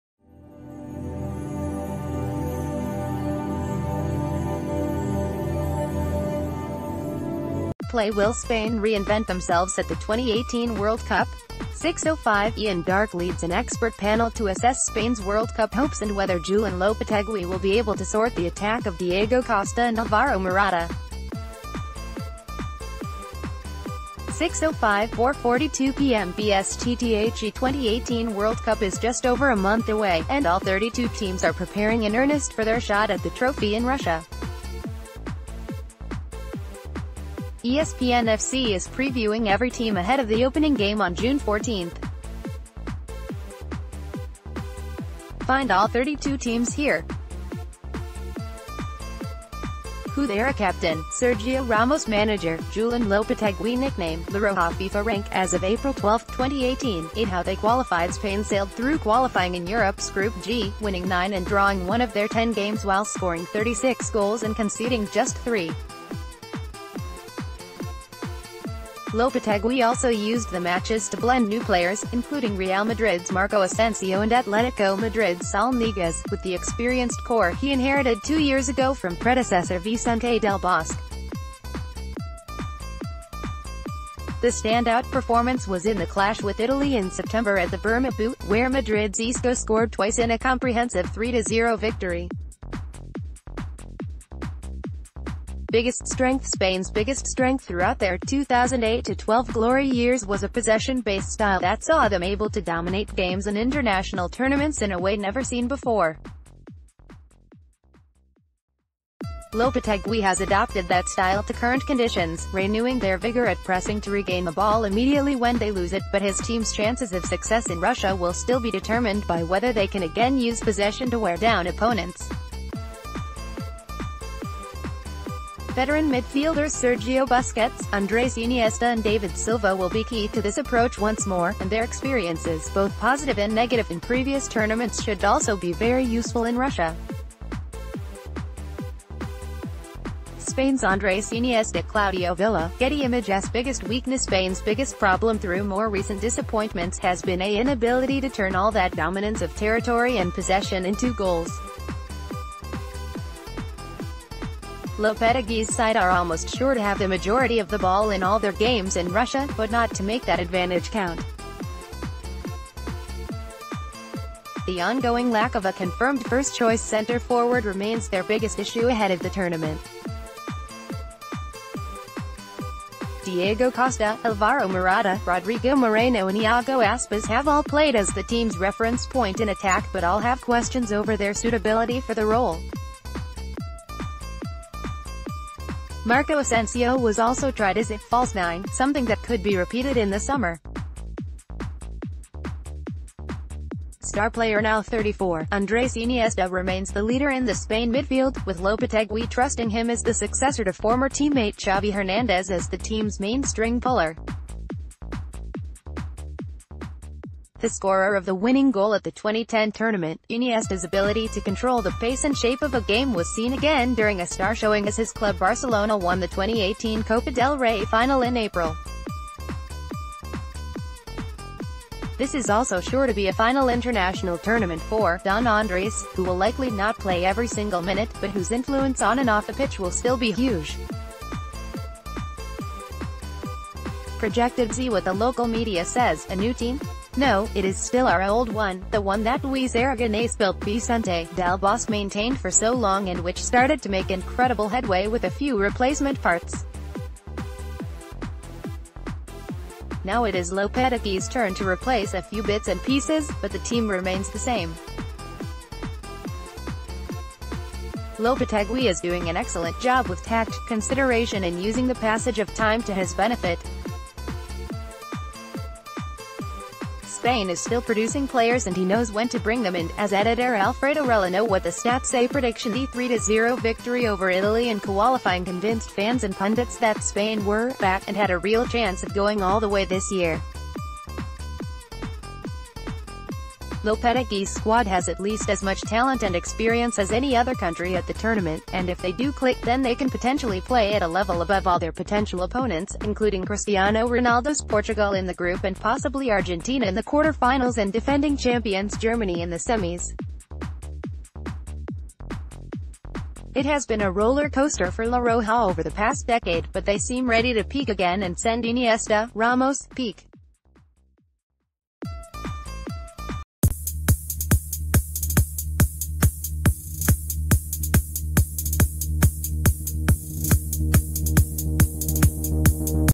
Breaking News | Spain, Andres Iniesta have talent, poise to reclaim World Cup in 2018
playWill Spain reinvent themselves at the 2018 World Cup? (6:05)Ian Darke leads an expert panel to assess Spain's World Cup hopes and whether Julen Lopetegui will be able to sort the attack of Diego Costa and Alvaro Morata. (6:05)4:42 PM BSTThe 2018 World Cup is just over a month away, and all 32 teams are preparing in earnest for their shot at the trophy in Russia. ESPN FC is previewing every team ahead of the opening game on June 14. Find all 32 teams here. Who they areCaptain: Sergio Ramos Ma...